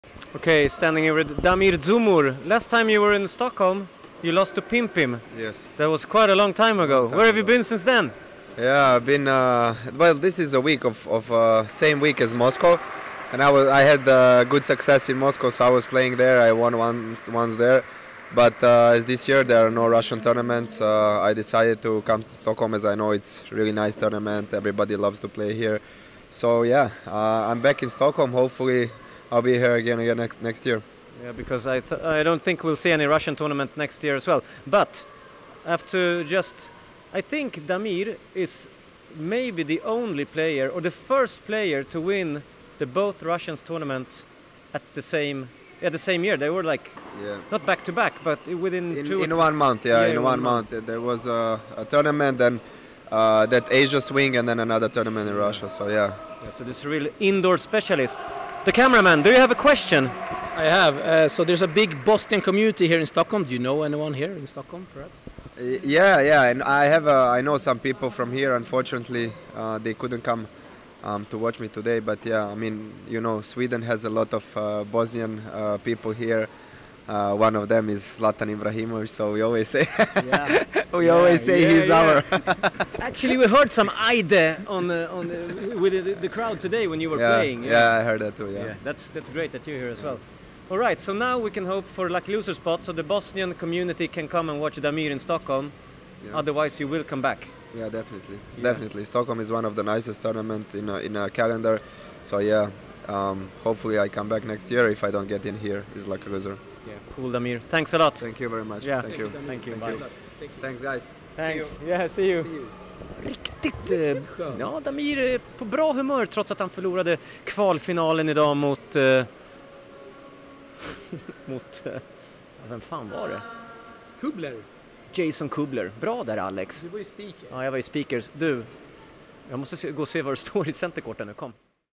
Interview with Damir Dzumhur: "Everybody loves to be in Stockholm"
Interview with Damir Dzumhur during Stockholm Open 2022.